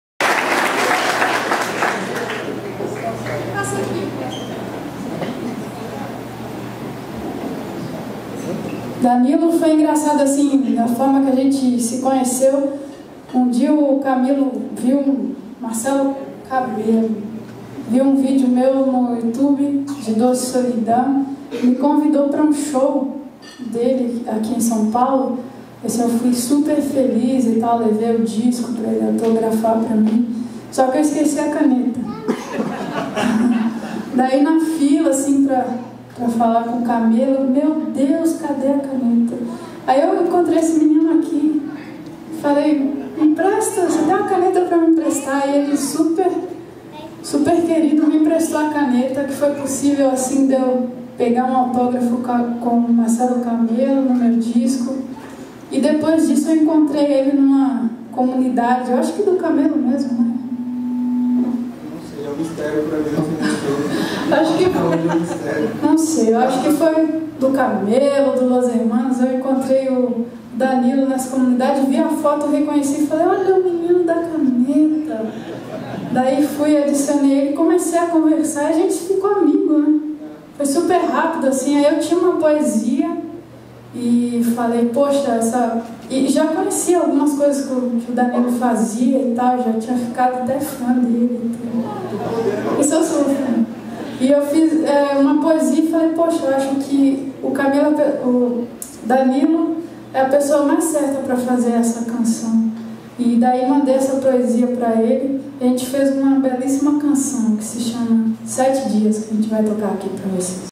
Roberta Campos [SEGUNDA NO CARMO] (11/2011) 10
at SESC Carmo, São Paulo, Brazil.

"A musicista mineira é um dos novos nomes que aparecem em um time de grandes cantoras da MPB. É fruto de um fenômeno do mundo moderno: lançou-se sozinha na Internet, fazendo fãs sem ser produzida profissionalmente, até atrair a atenção de empresas do segmento. Durante sua carreira, obteve grandes conquistas como já ter cantado com Milton Nascimento e Paulinho Moska. 196 lugares."